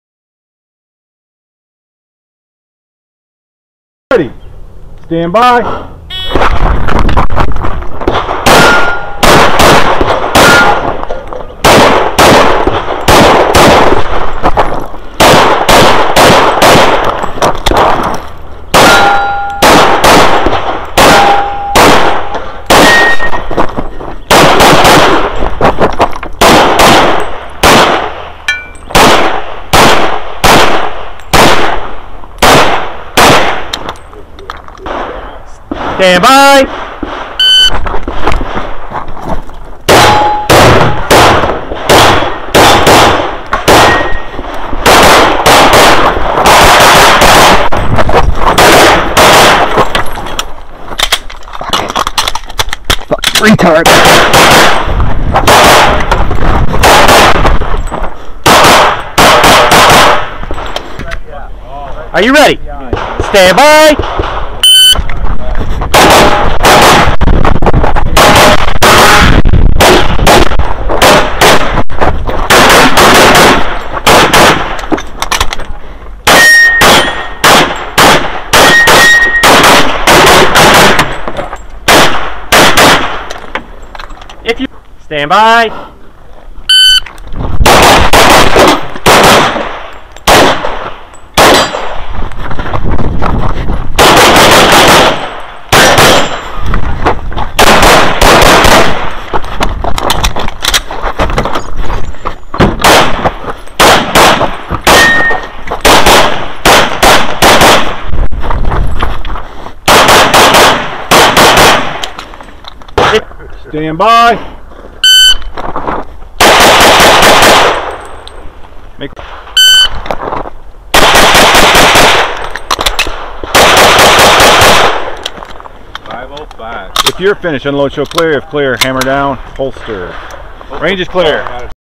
NPCCC July 2016 USPSA
My shooting was good, other than forgetting when to reload on a stage and getting fucked by a retarded forward falling popper that bounced back up and blocked the mini popper behind it.